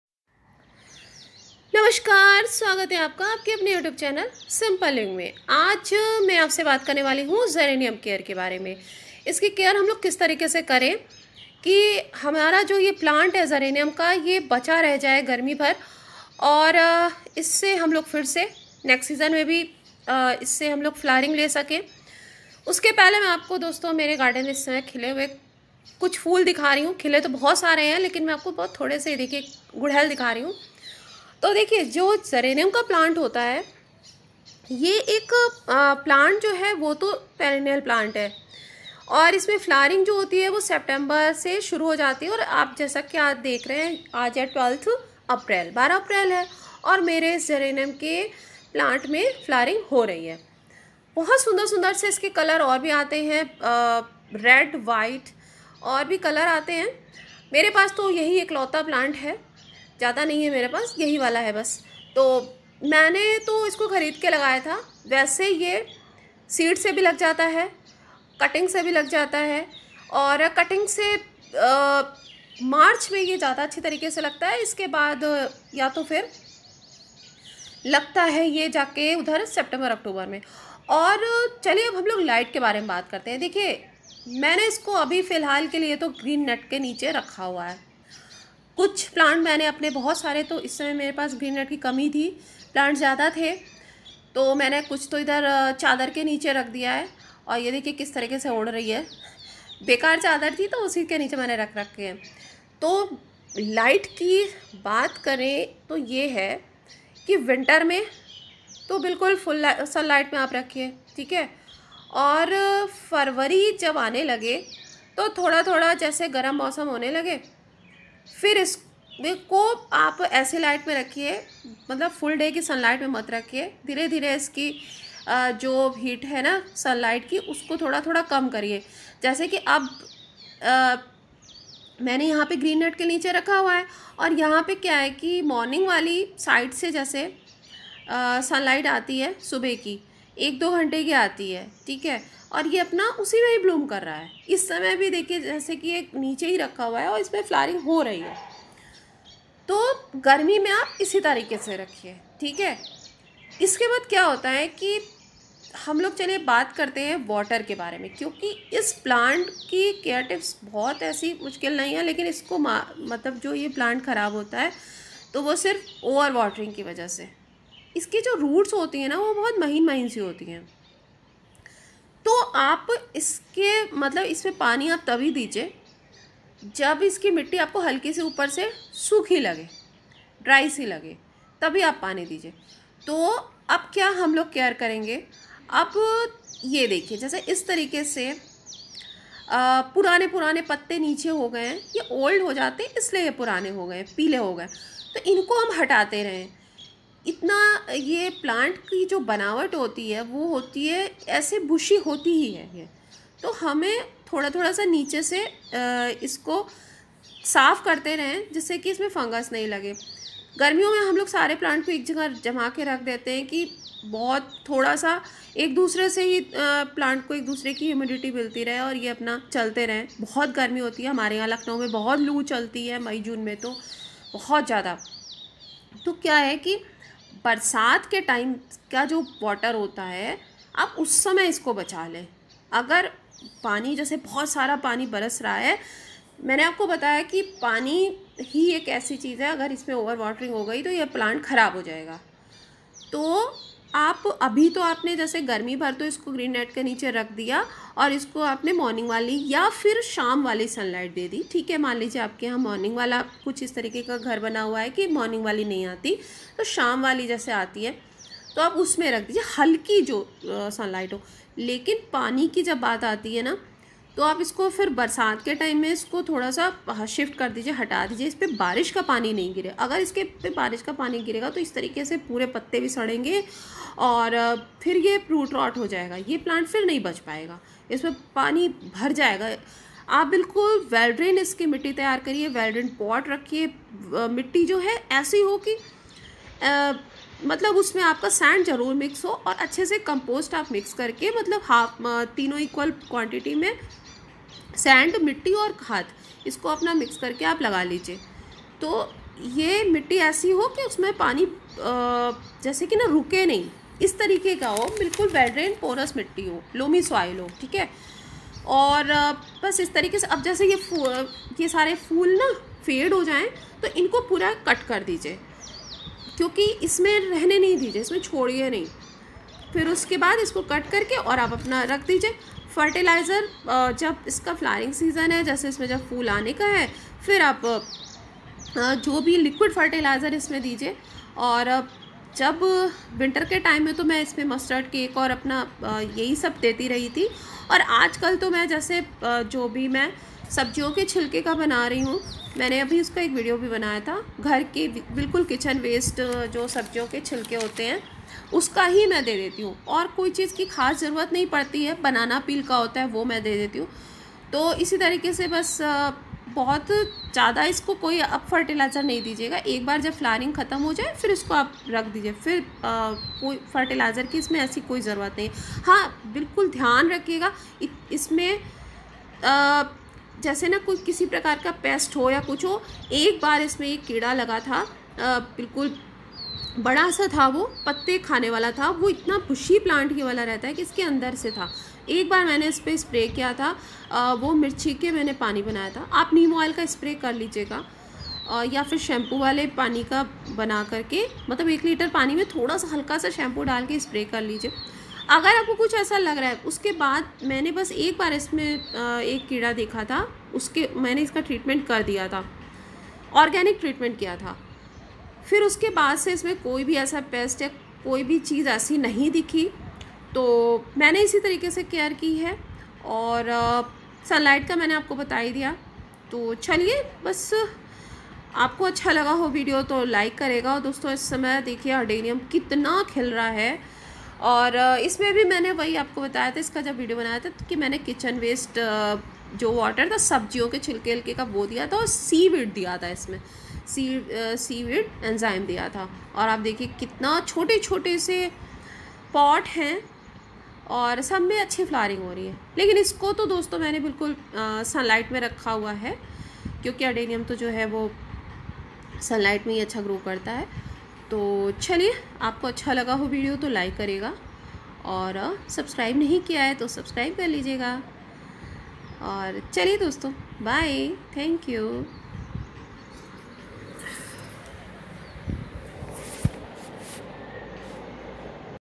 Save Care Geranium Flower plant in Summer India ♨️ गर्मी में जरेनियम की देखभाल hindi
how to save and care Geranium flower plant in summer